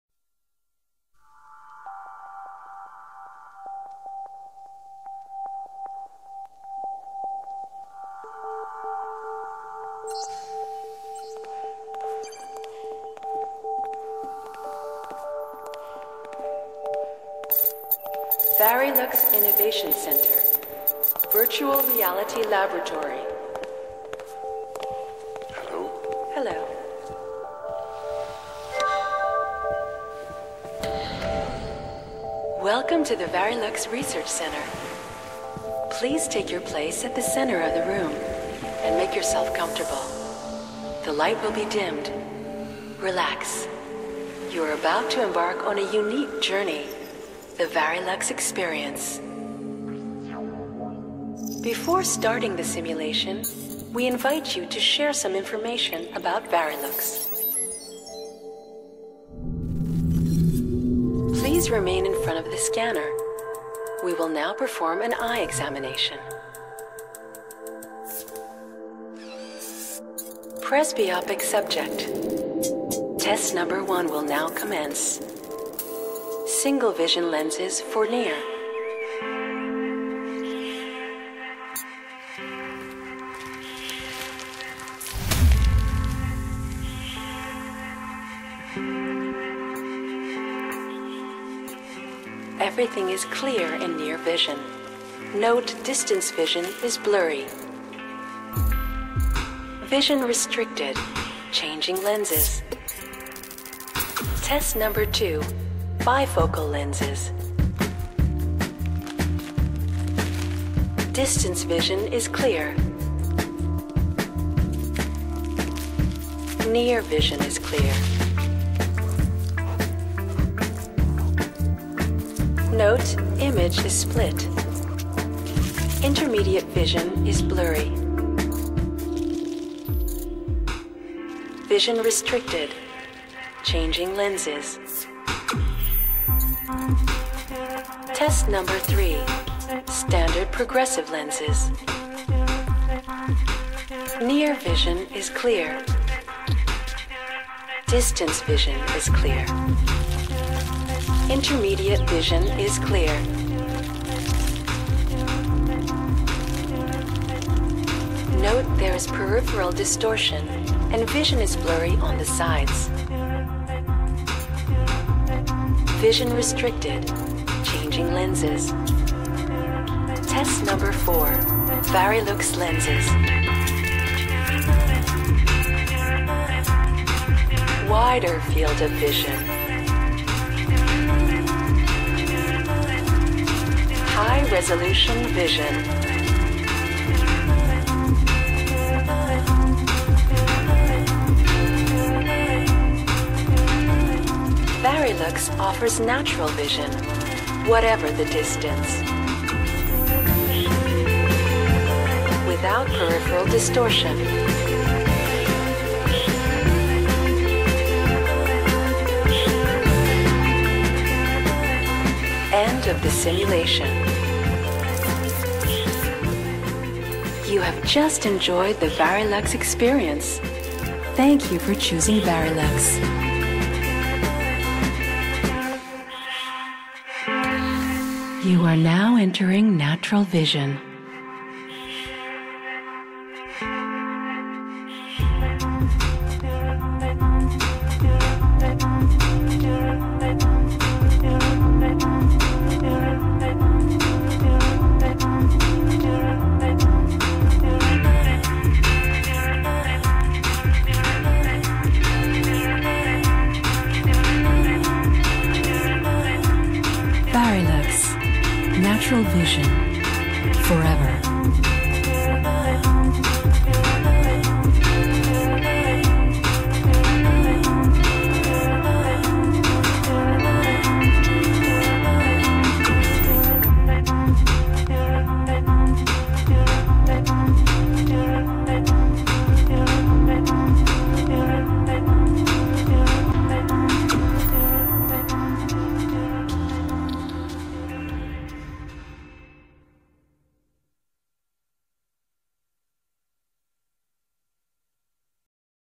Tipos de Lentes
Demo de los tipos de lentes y como funcionan. estos lentes los encuentras en OPTICAS ACUARIO